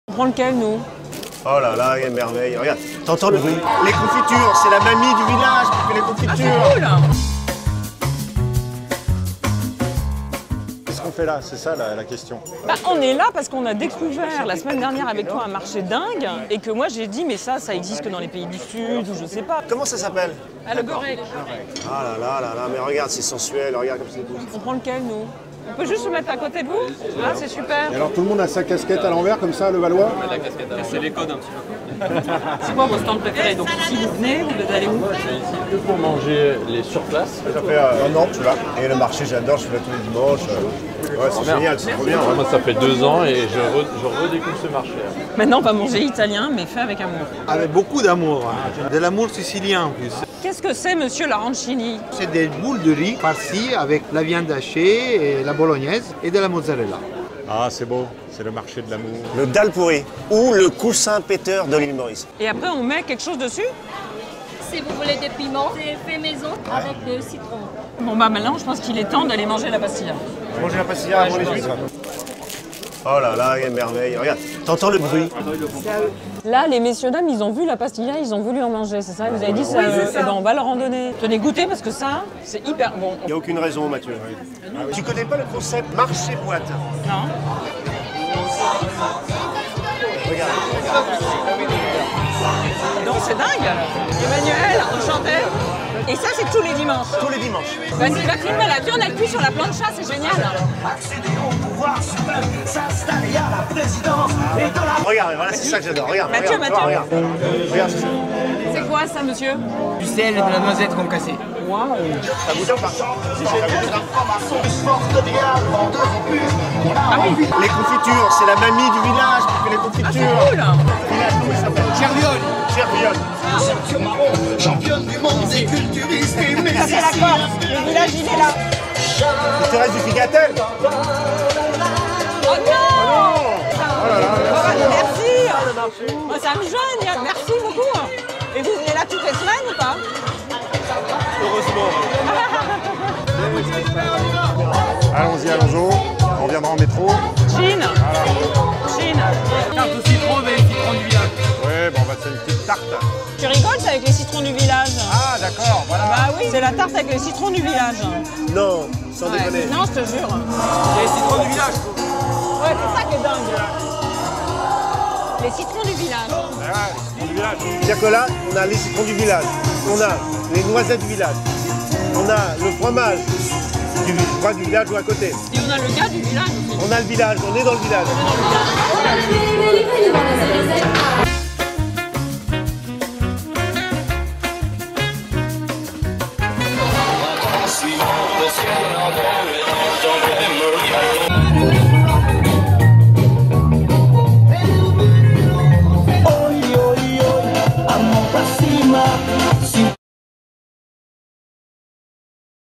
Marché de Levallois
C'est meilleur quand c'est bon #48
Ça se passe à LEVALLOIS-PERRET aux portes de Paris et c'est fou fou fou, pour déguster fromages et charcuteries corses, pastilla marocaine, arancini italiens... et s'amuser.